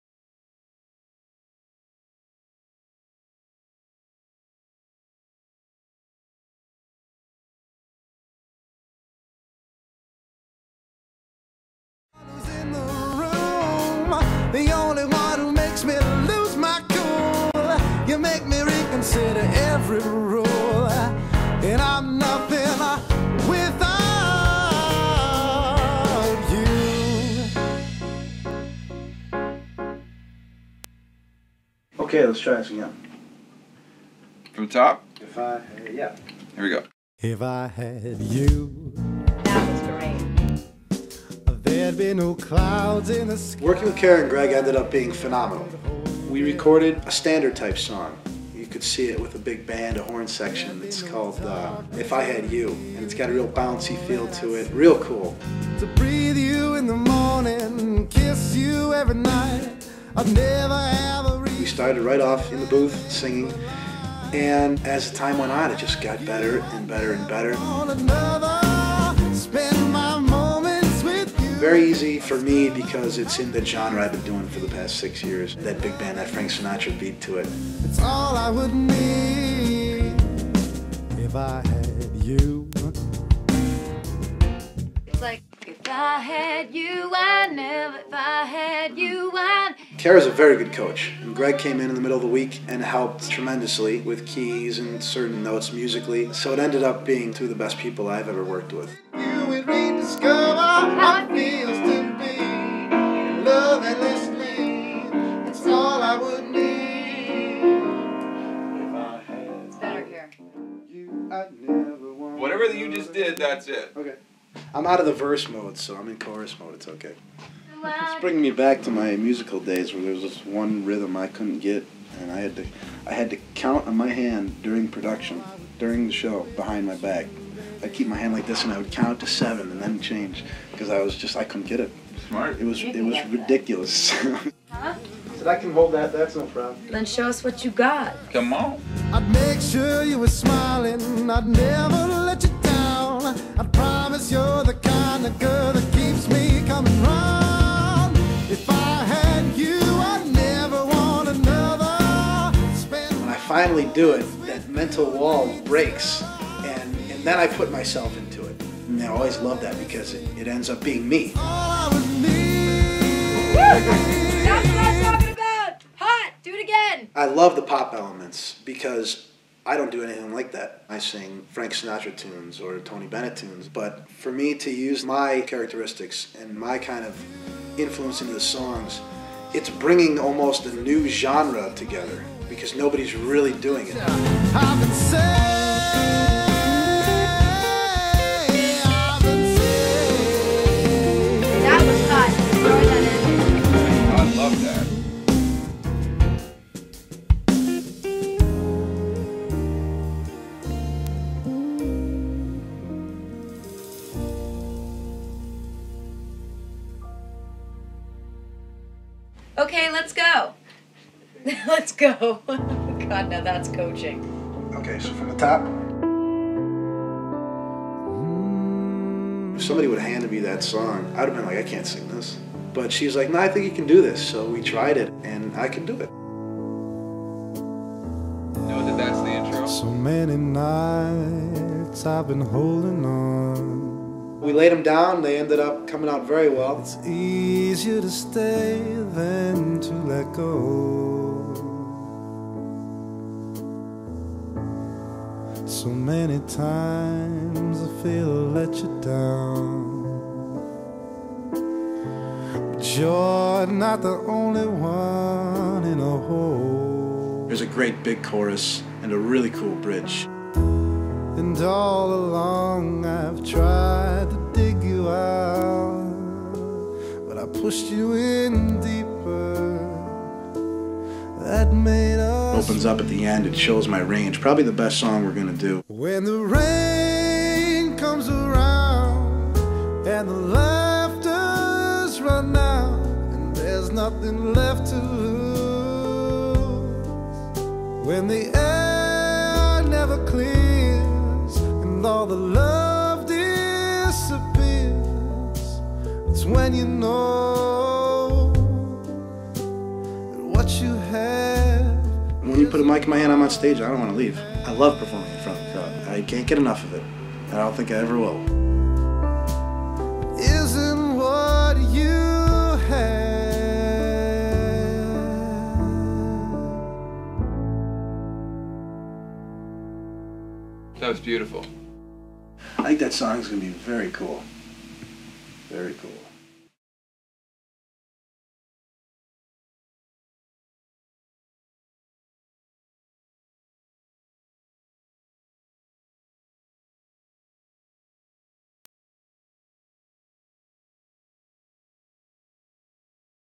New Kara DioGuardi Video!!!
PAUL SAFY JR Works with KARA DIOGUARDI and Greg Wells Back In 2005, Kara, wrote 3 songs for Paul